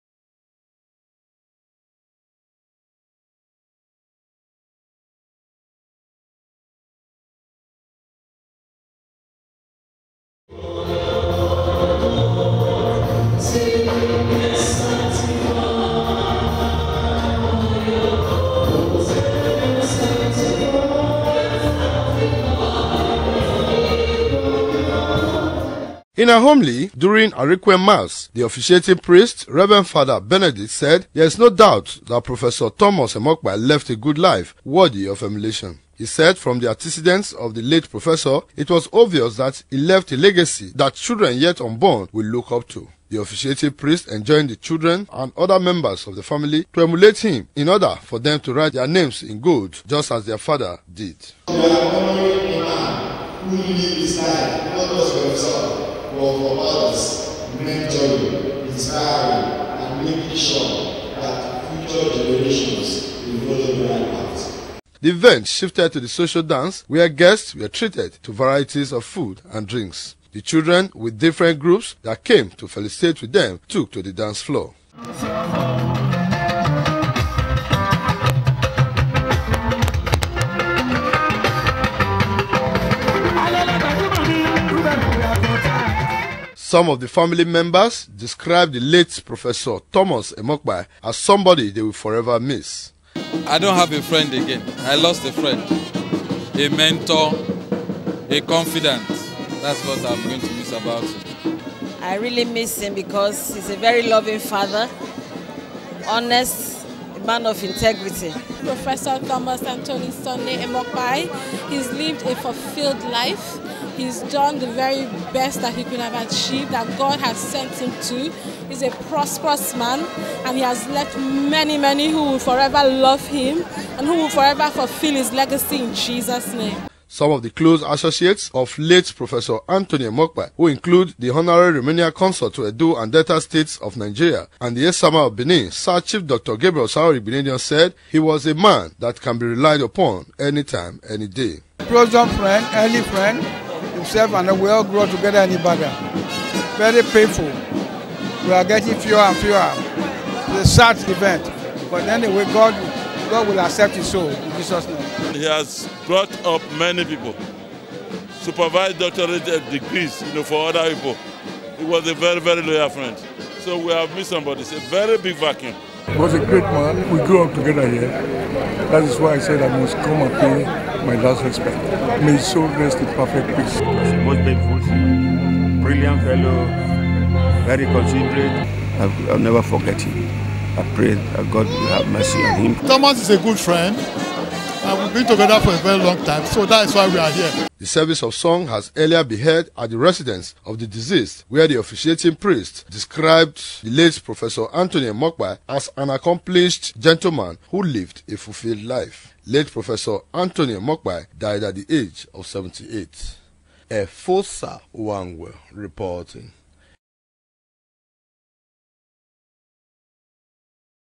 Funeral Rite Of Prof. Emeritus Anthony Emokpae Held In Benin
The funeral rite of Professor Emeritus, Thomas Anthony Emokpae has been held amidst fun fare in Benin.